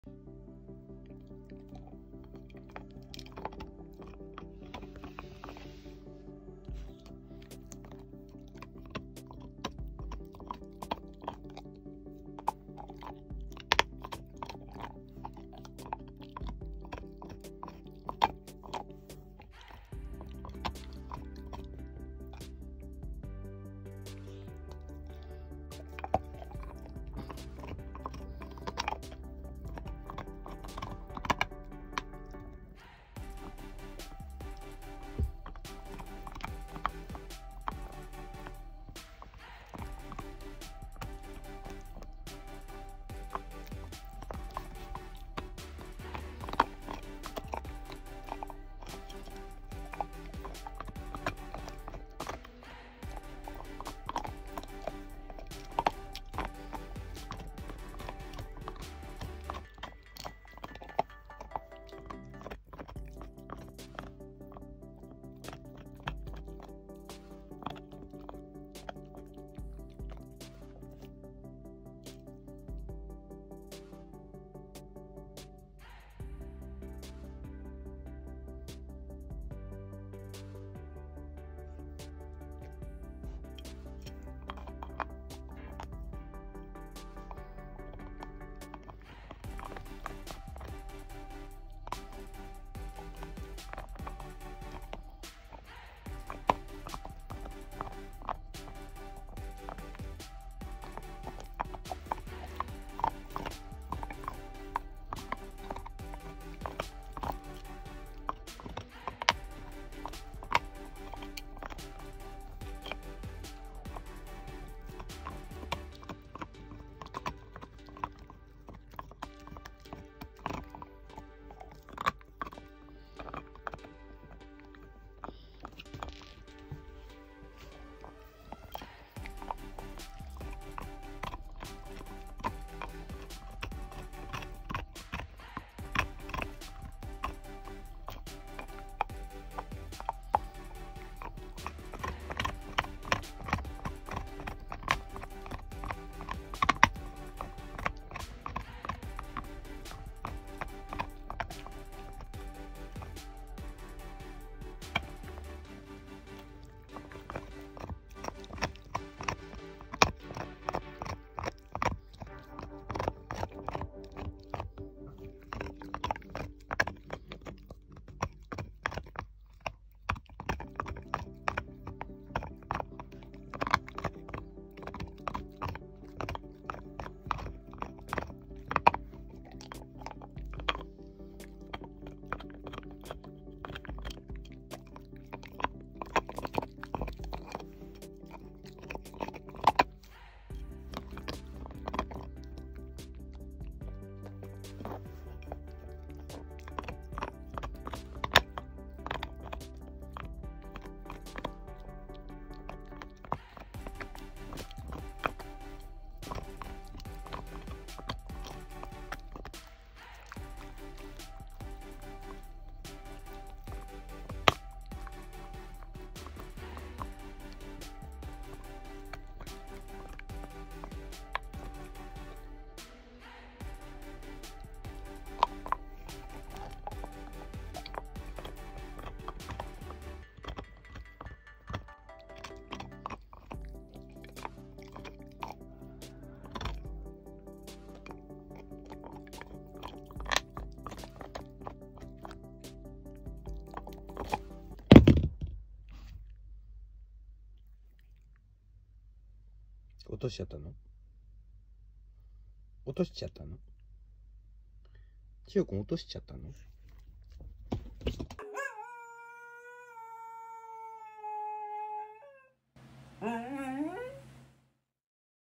【歯がきれいな理由】毎日飽きもせずガリガリするイングリッシュコッカースパニエル【ちよ君】癒し
This is the reason why his teeth are beautiful.
ちよ君は歯垢がない。そりゃそんだけ固いのかじってたらね。かわいい。